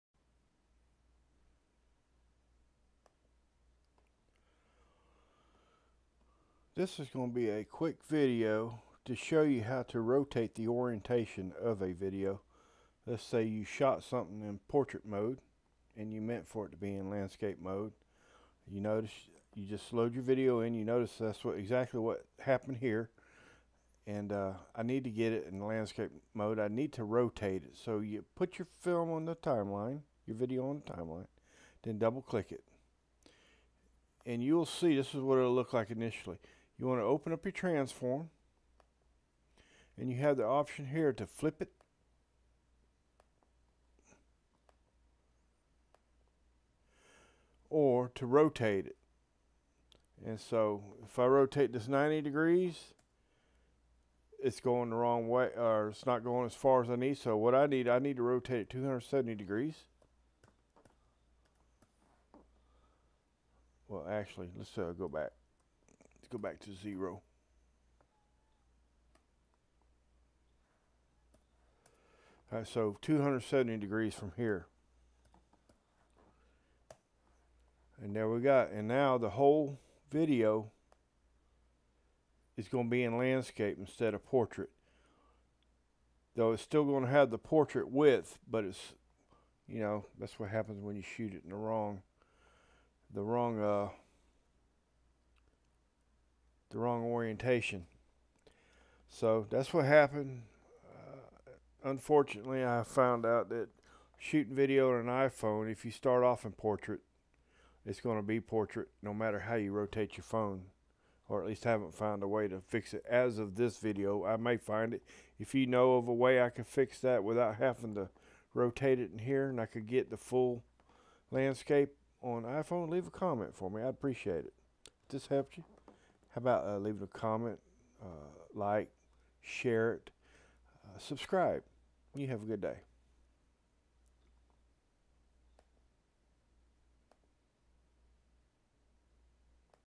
Rotate or Flip Video Orientation in Filmora
Did you shoot your video in portrait mode when you meant to shoot it in landscape mode or visa versa? When editing your video in Filmora you can rotate or flip the orientation of your video. This is easy to do and this short video tutorial will show you how to rotate or flip video on the timeline in Filmora.

Learn Video Editing in Filmora Tutorial